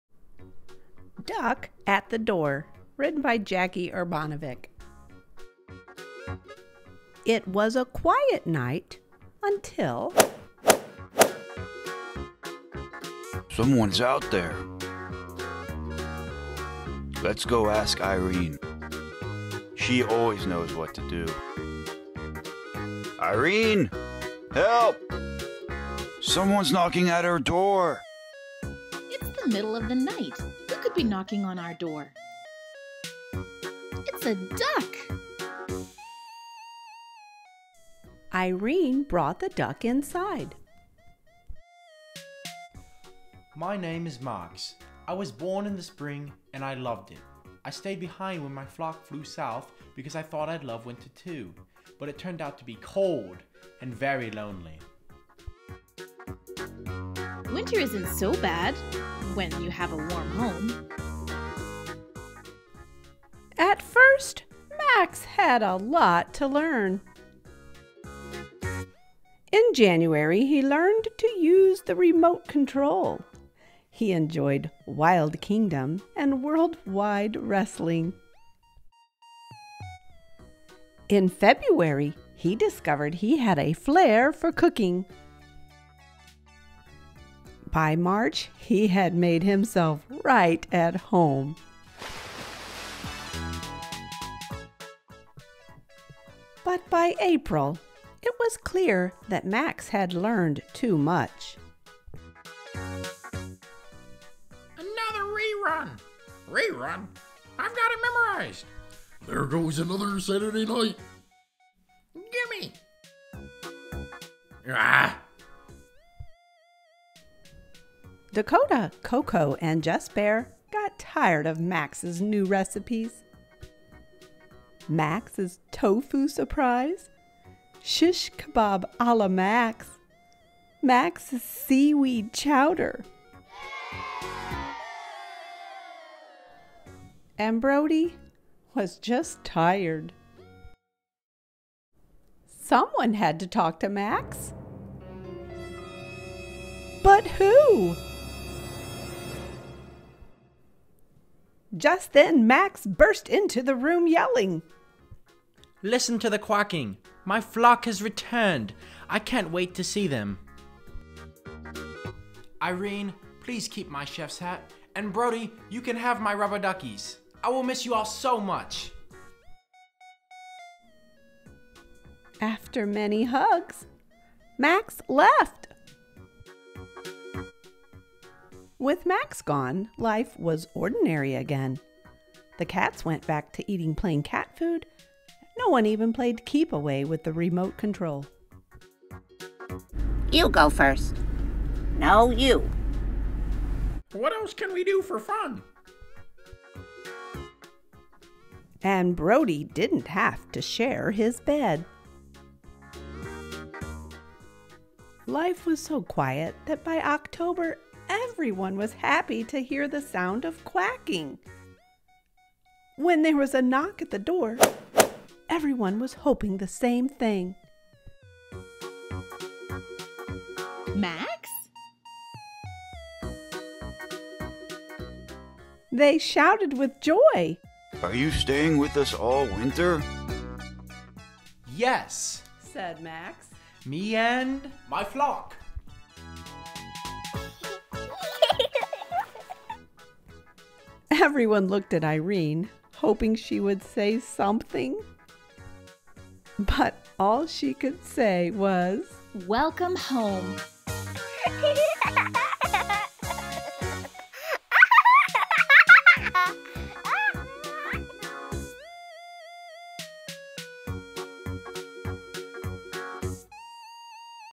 Duck at the Door | Voice over collaboration
Ms Kathy joins four other youtube creators in a collaboration to bring this story to life.  Duck Soup was written by Jackie Urbanovic.

Ms Kathy narrates and her son Michael helped with voices too.